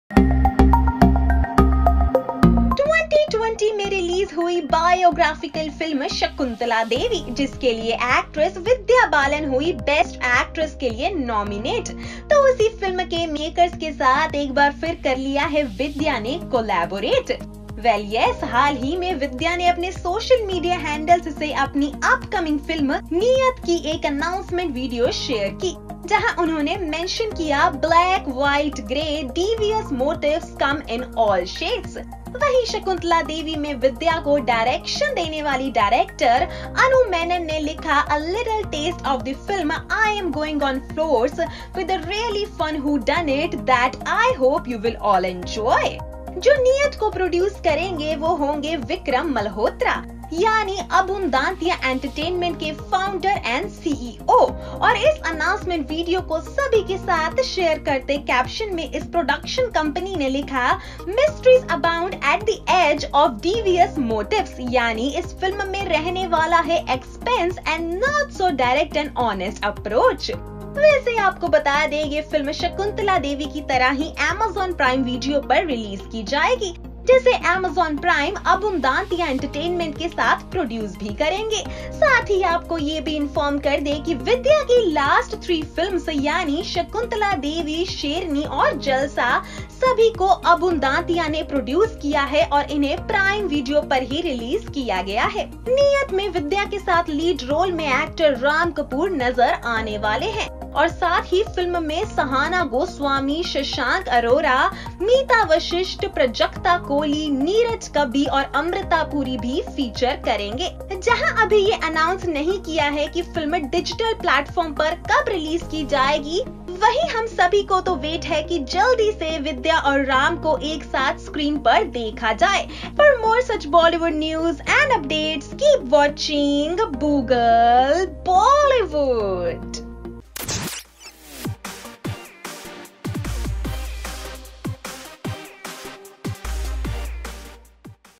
Ram Kapoor to be paired opposite Vidya Balan in this film I Boogle Bollywood
Copyrights@BoogleBollywood